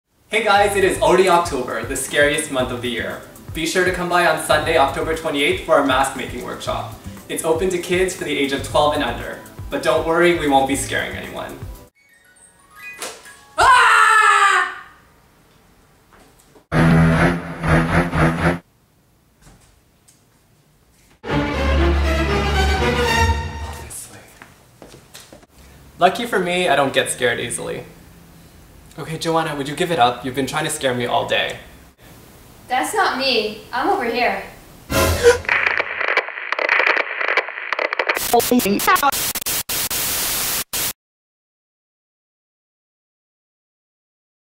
Halloween Mask Making Event!
TUESDAY TOY TALK:  It's already October, and that means Halloween is right around the corner!  Be sure to come participate in our fun mask making event!  Trick or treat!